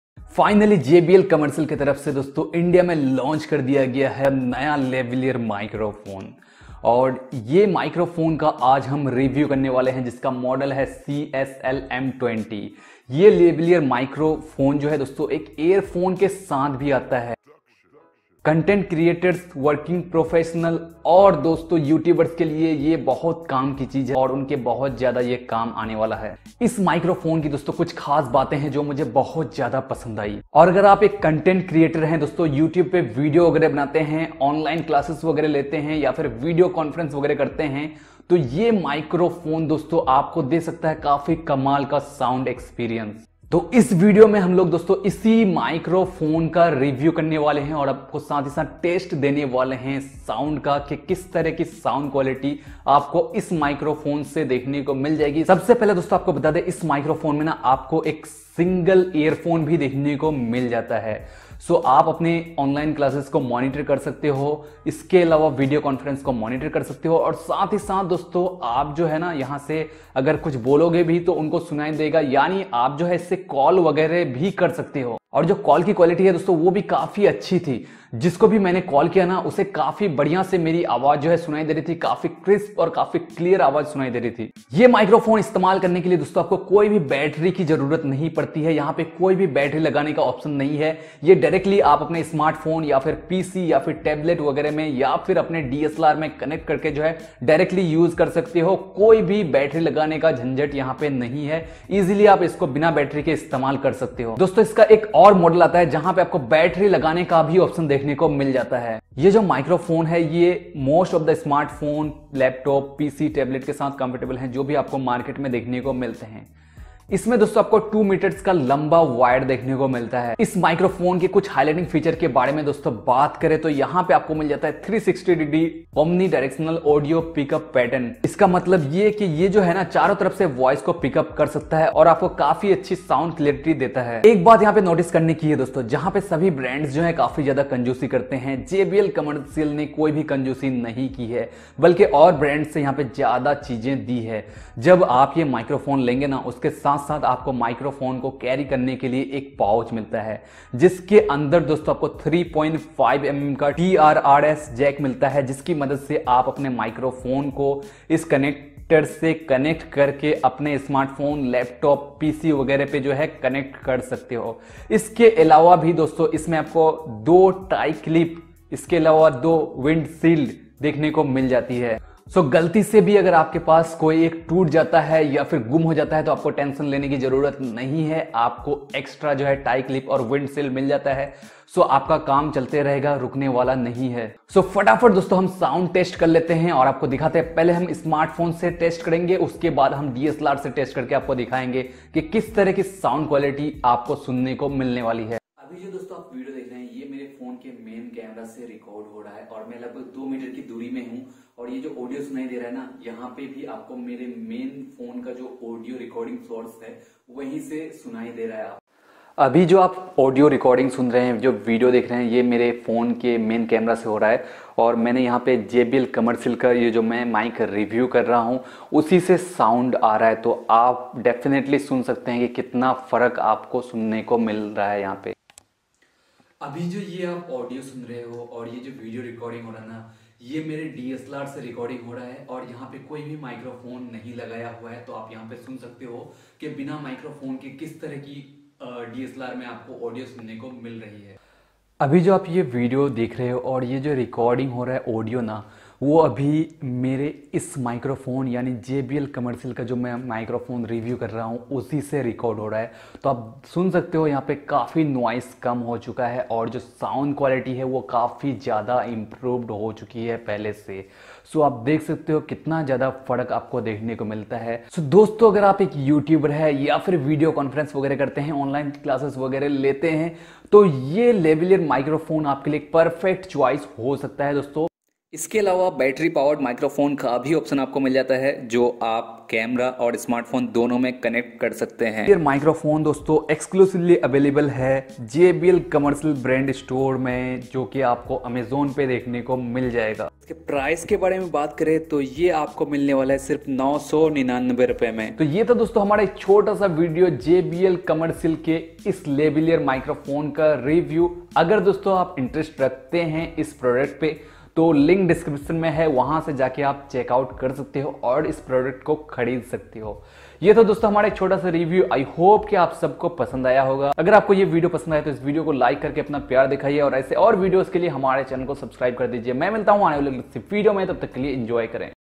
M Talks' Review of the JBL Commercial CSLM20 & CSLM20B microphones
Check out this review of the new JBLCommercial​​ mics by M Talks!